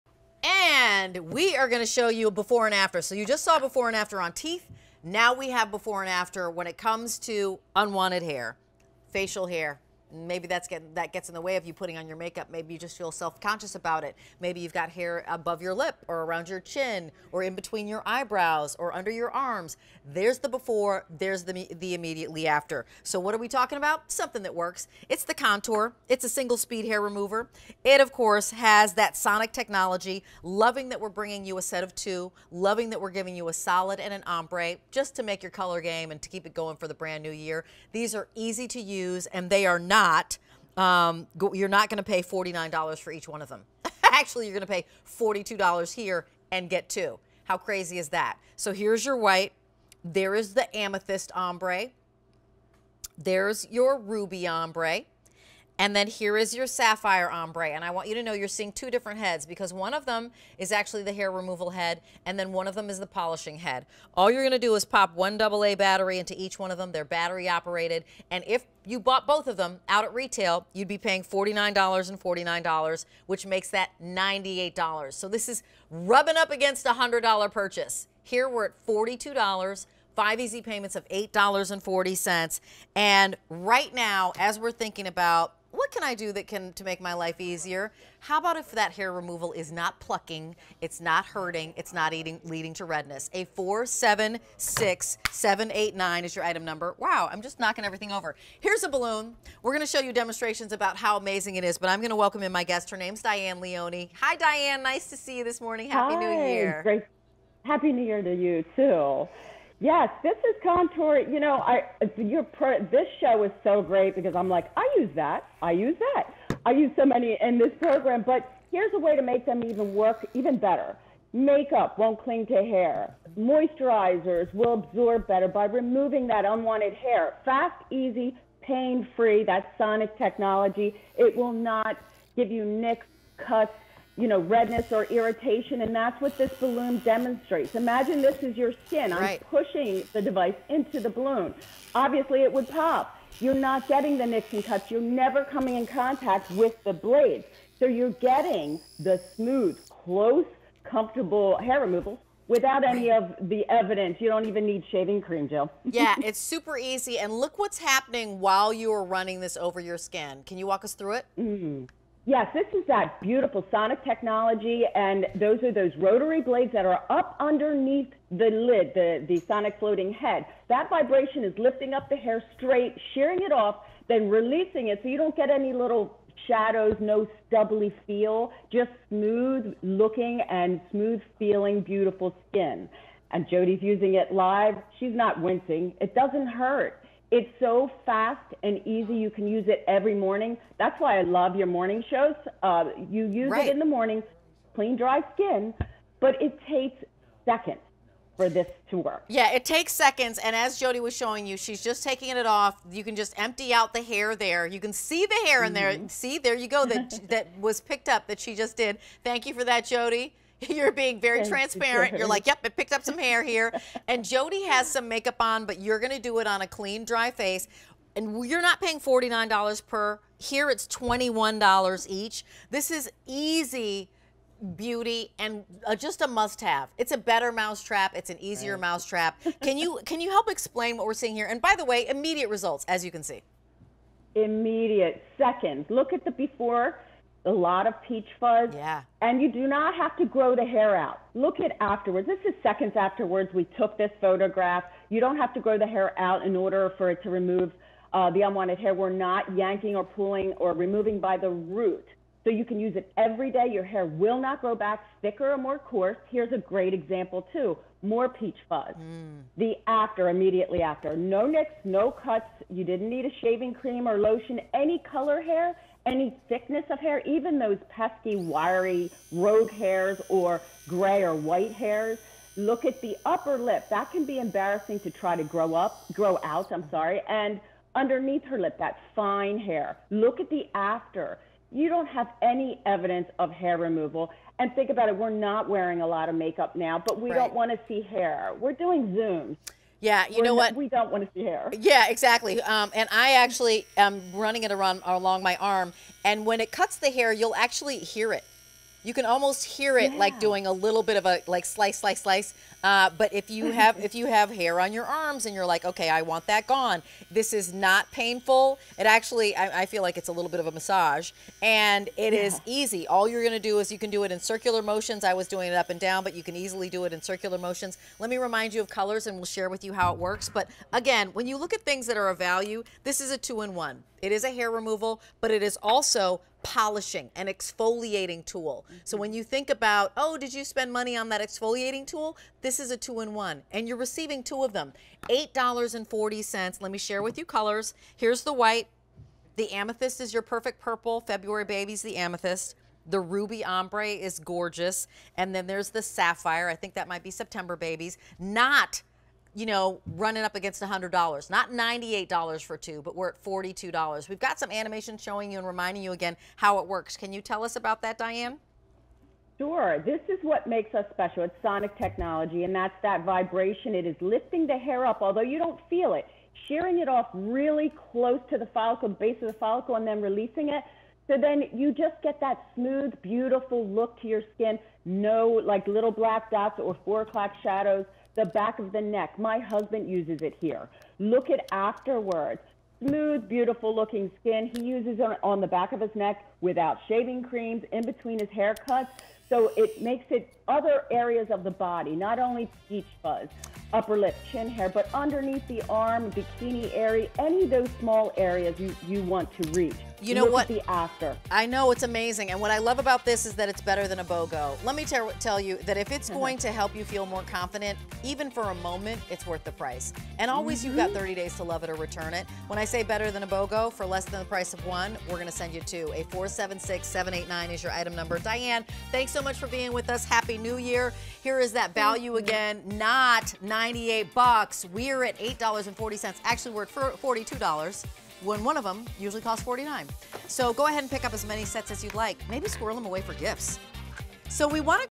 Conture Single Speed Hair Remover Tools Set of 2 on QVC
Conture Single Speed Hair Remover Tools Set of 2

The Conture hair removing device is designed to help remove facial hair and exfoliate the surface of the skin, revealing flawless and youthful-looking skin. Receive (2) Conture Hair Remover Tools